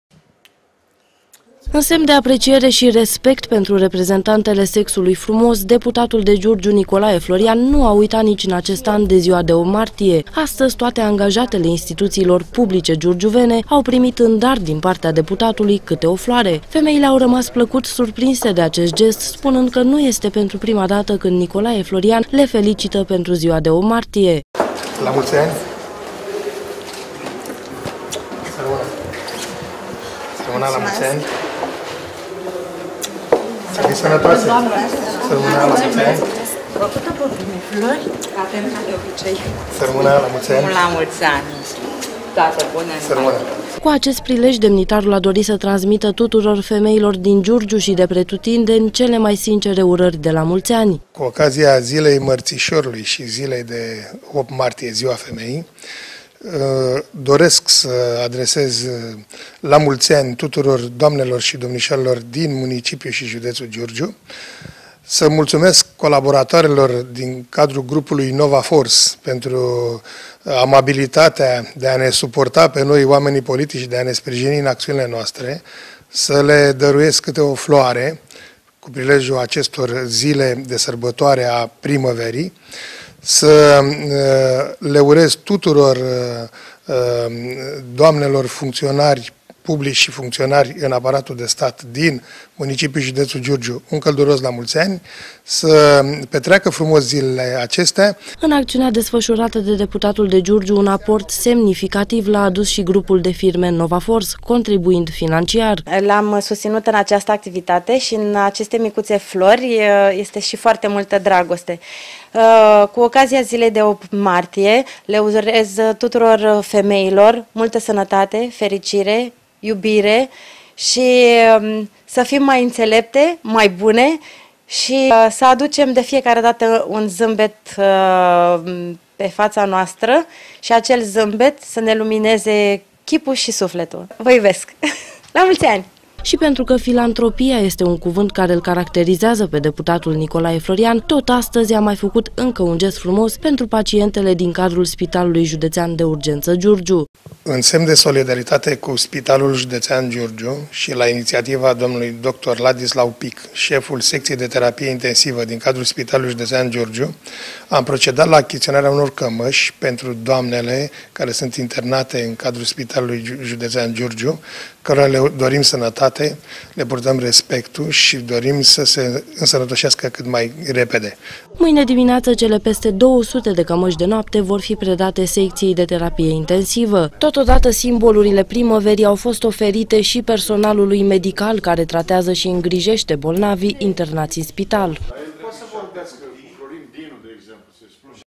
GESTURI FRUMOASE DE 8 MARTIE
Ca în fiecare an, deputatul de Giurgiu, Nicolae Florian, nu uită că ziua de 8 Martie este o zi specială pentru femei. Sprijinit de grupul de firme Nova Force, demnitarul a oferit astăzi flori, tuturor angajatelor din instituțiile publice giurgiuvene.